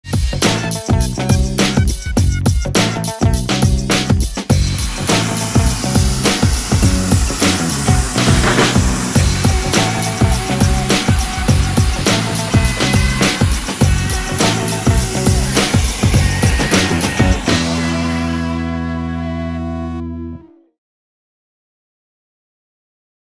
Why use PRO F HD 1080p
New 2021 Torgos T35 road drying machine with PRO-F nozzle. Cemar.pro